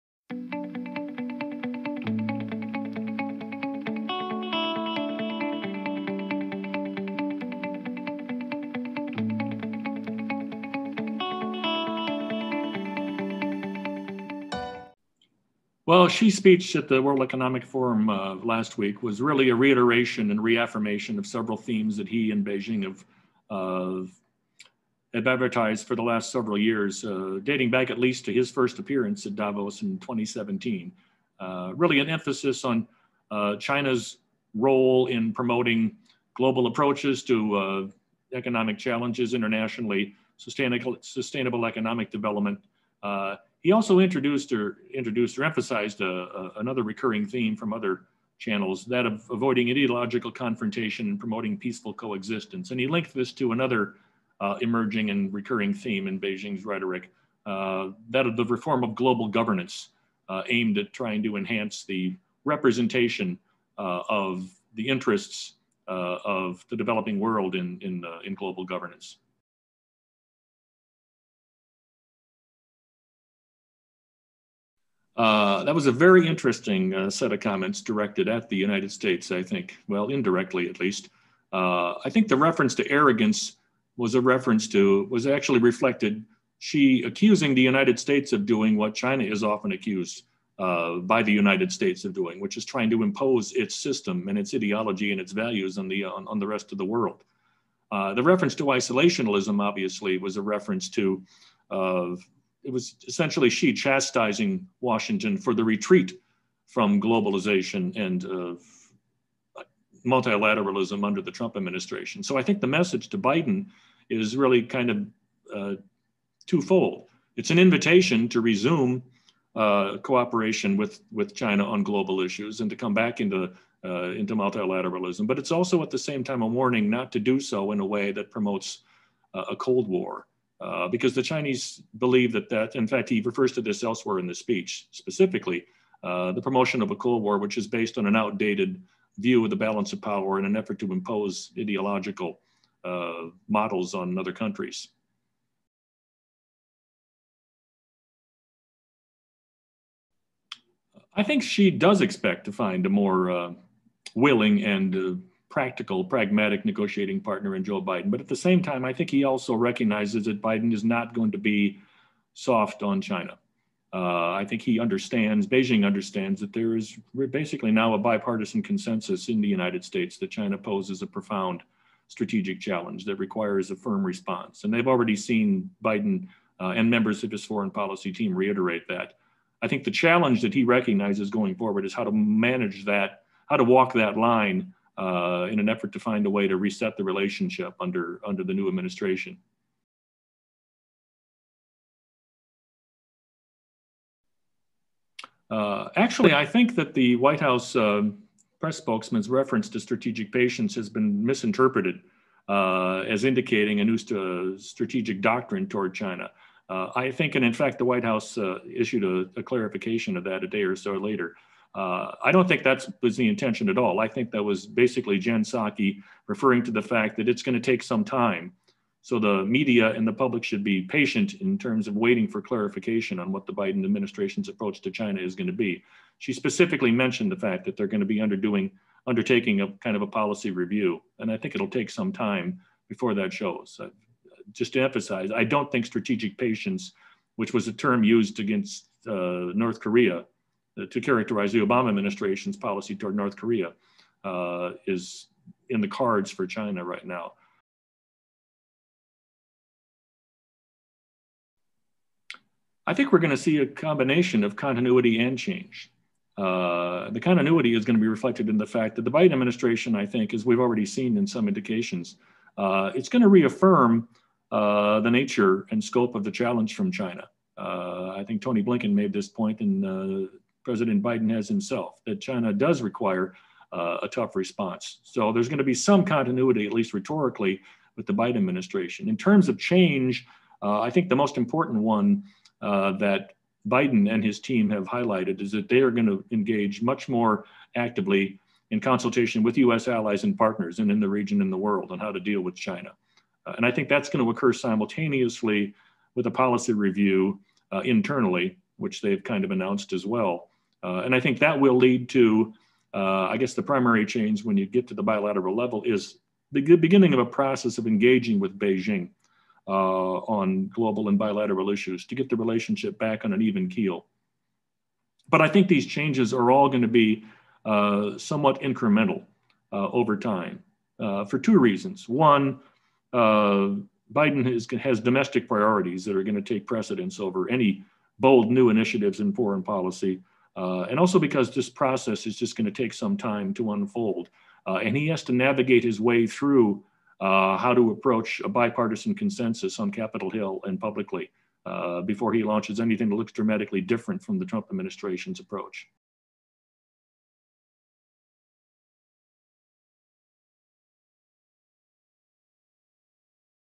Unobstructed Views - Xi Jinping and the World Economic Forum 2021
This week,  Xi Jinping addressed the World Economic Form. Paul Heer explains what Xi's remarks mean for Biden's administration and what we can expect from U.S.-China relations going forward.